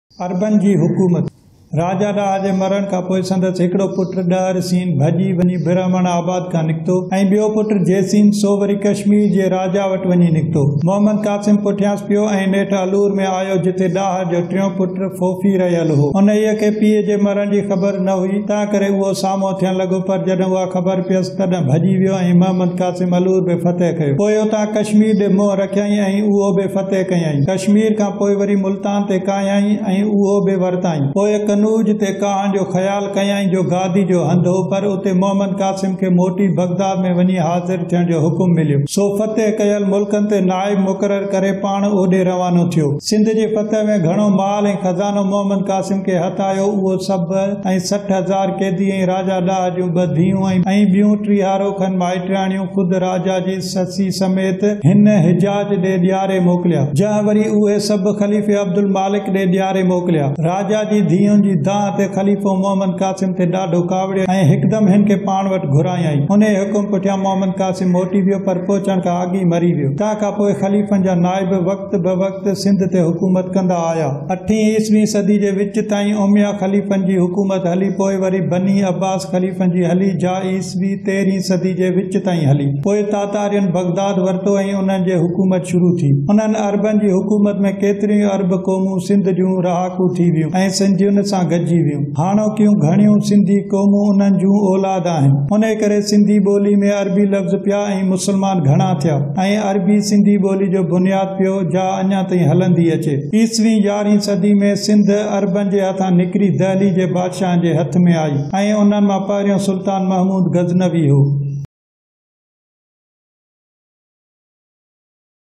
Araban Ji Hakoomat|Mirza Qaleech Baig|Part_8 |عربن جي حڪومت|مرزا قليچ بيگ|حصون اٺون
ليکڪ پاران
پڌرو هجي ته سنڌ جي وڏي تاريخ، تصويرن سان، سنڌي ۾ مون اڳ ئي لکي آهي ۽ سنڌ جي تمام مختصر تاريخ مُلن جي مڪتبن لاءِ به اڳ  ئي لکي اٿم. پر وچولي قسم جي مختصر تاريخ سنڌي  ننڍن درجن ۾ پڙهائڻ لاءِ گهربل هئي. انهن لاءِ مون هيءِ سنڌ جي تاريخ سبقن ۾ لکي آهي. جن جي پڙهڻ مان ننڍن پڙهندڙن کي مزو به ايندو ۽ سنڌ جي ڳنڍيل تاريخ به معلوم ٿيندي.

قليچ بيگ
آڪٽوبر 1916ع
حيدرآباد